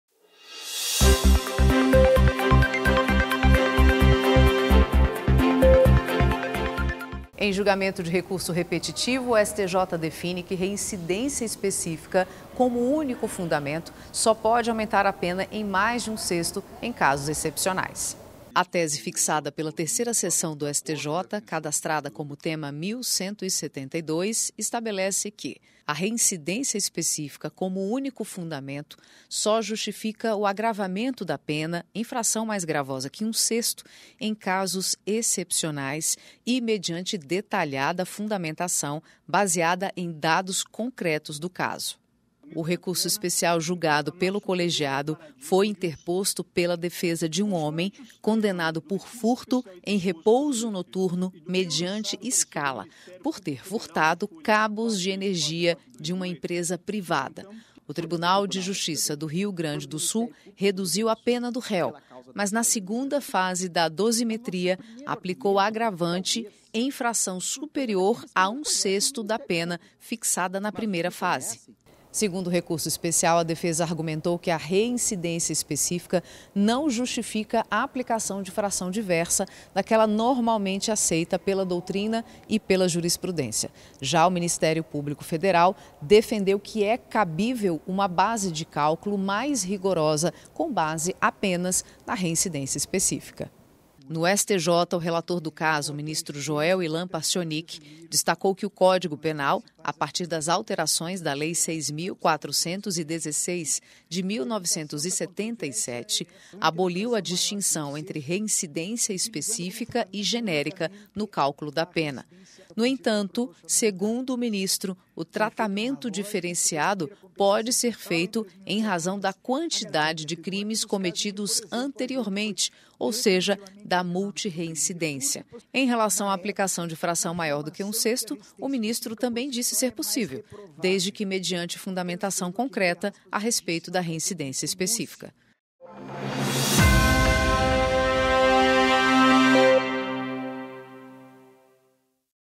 Reincidência específica como único fundamento só aumenta pena em mais de um sexto excepcionalmente
​A Terceira Seção do Superior Tribunal de Justiça (STJ), sob o rito dos recursos repetitivos (Tema 1.172), adotou a seguinte tese: "A reincidência específica, como único fundamento, só justifica o agravamento da pena em fração mais gravosa que um sexto em casos excepcionais e mediante detalhada fundamentação baseada em dados concretos do caso".